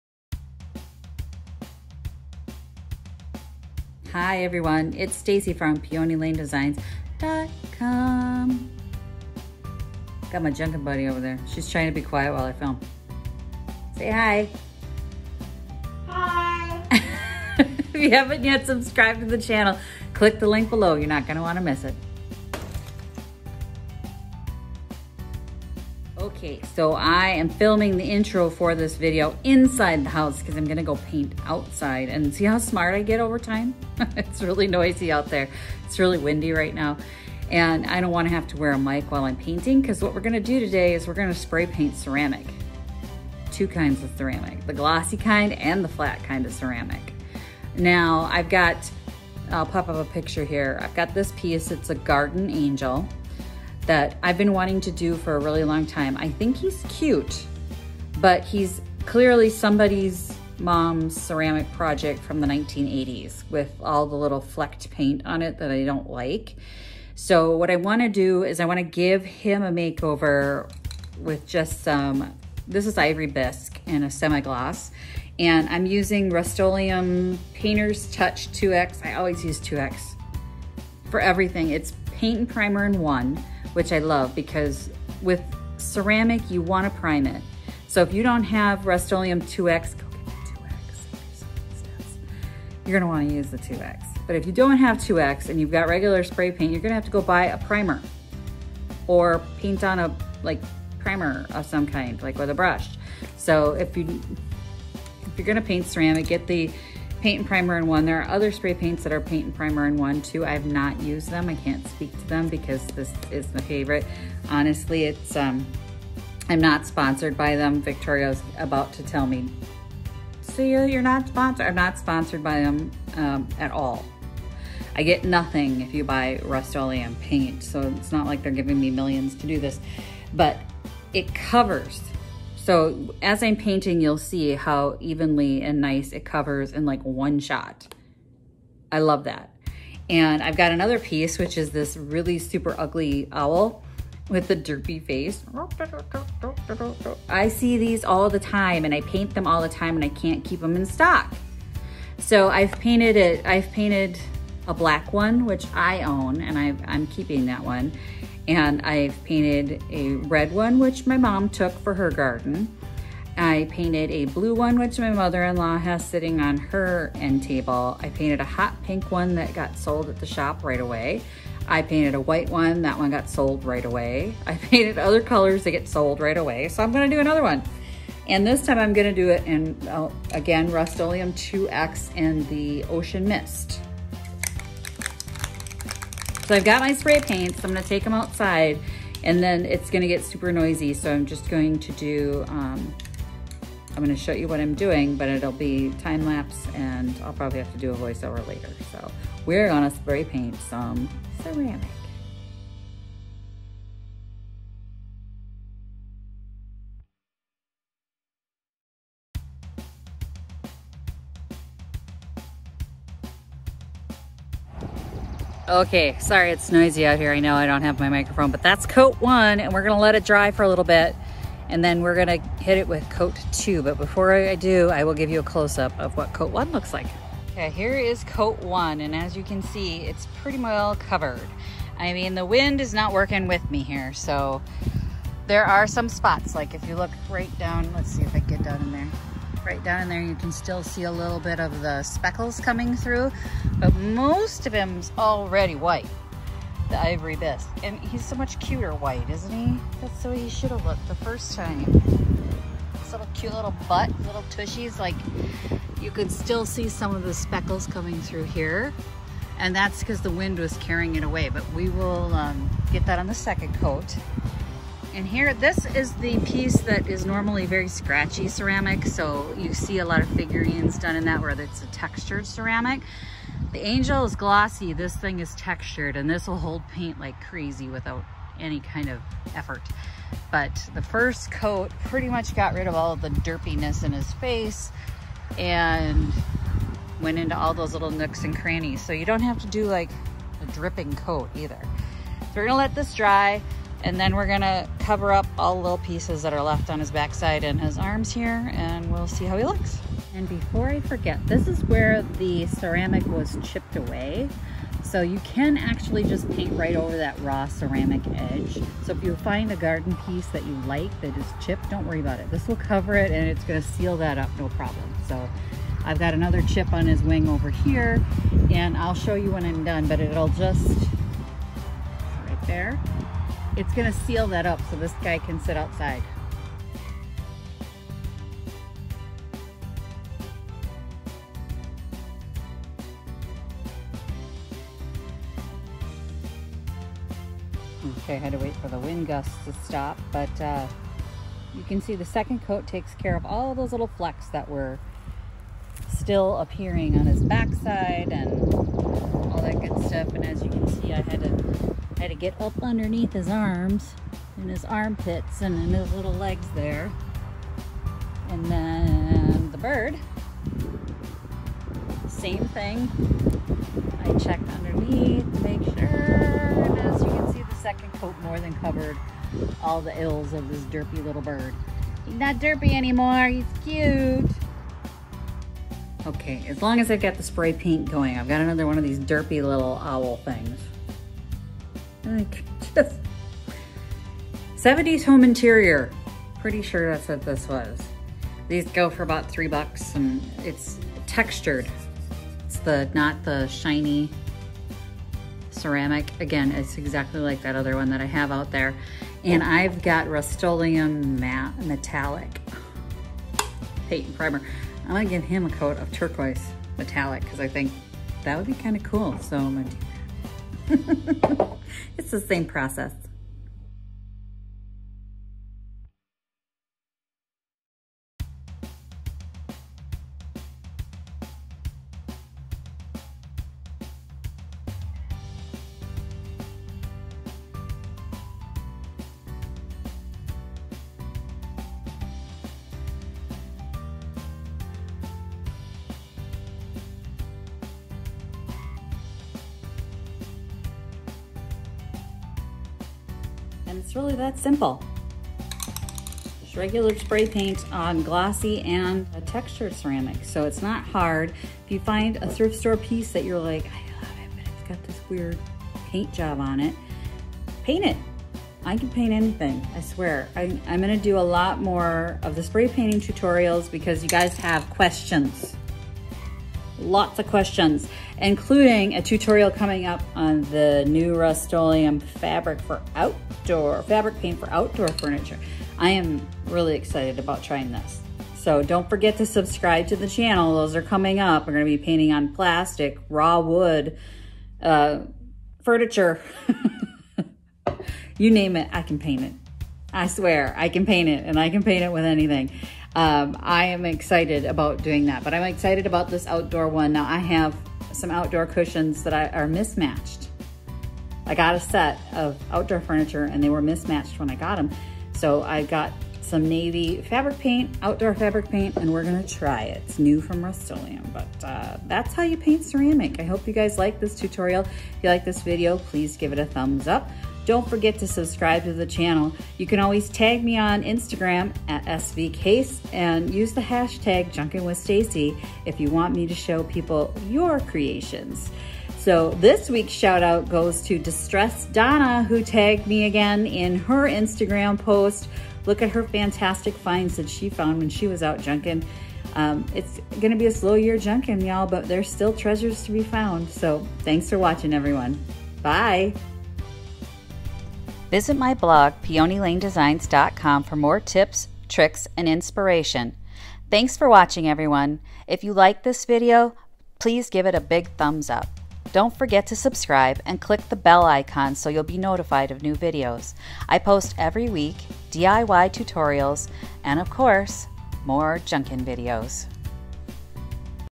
Spray Painting Ceramic
Spray Painting Ceramic - don’t pass up that weirdly painted, gaudy ceramic garden statue! You can easily spray paint it!

Send coffee! I’m on half a tank!
Want to send me something?
Stacy Verdick Case
Thrift Flip